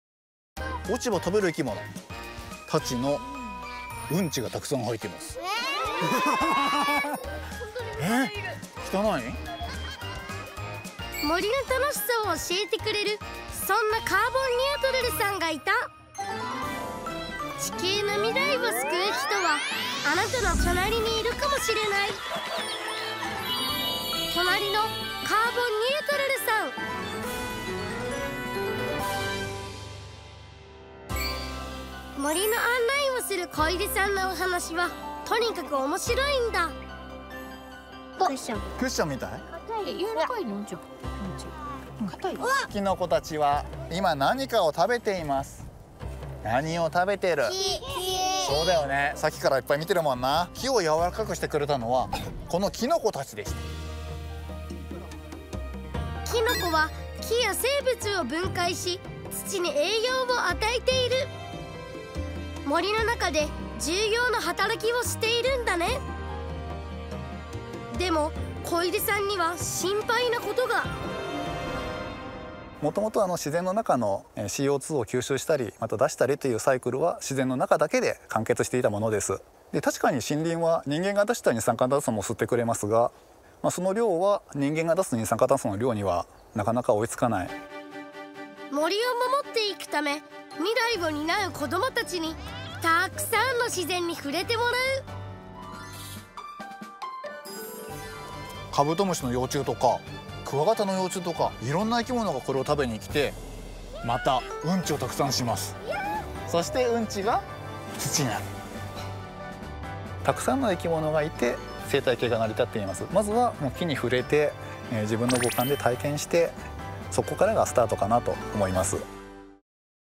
#34 子どもたちがキノコで学ぶ！（となりのカーボンニュートラルさん）
森についてとっても詳しいカーボンニュートラルさんがいた！木や生物に触れてもらいながら子どもたちに教えているのは森を守るための大事なこと。

ナレーション：加登ななひ